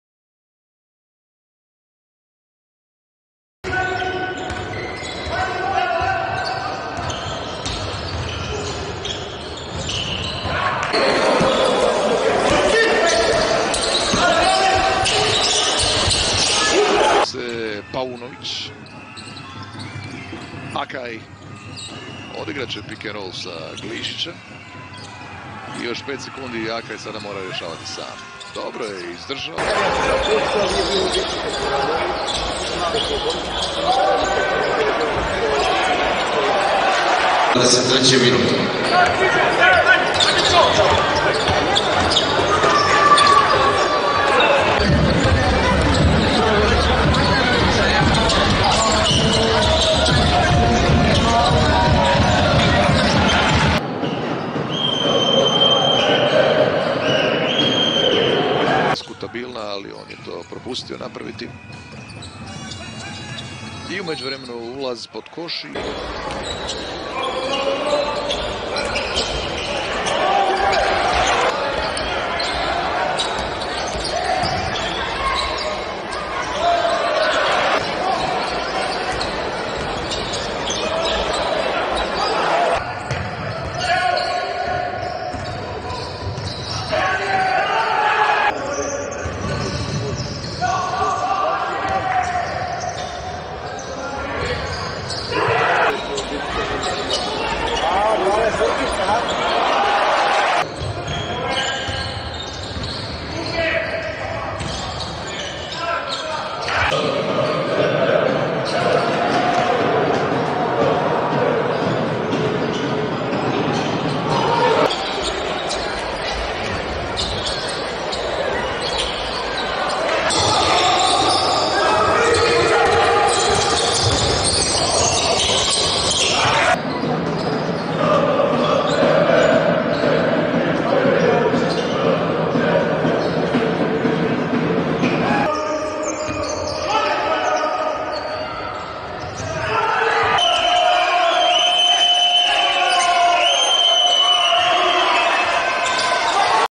Mert Akay - KK Dynamic BEOGRAD 19/20
SERBIA-KLS - 17 GAMES / MIN:20,4 / PTS:8,5 / 2 PTS %52,3 / 3 PTS %23,3 / FT %73,4 / R : 3,4 / ASSIST :5,5
ABA LEAGUE - 13 GAMES / MIN:18,3 / PTS:5,3 / 2 PTS %42,9 / 3 PTS %14,3 / FT %51,7 / R : 2,5 / ASSIST :4,2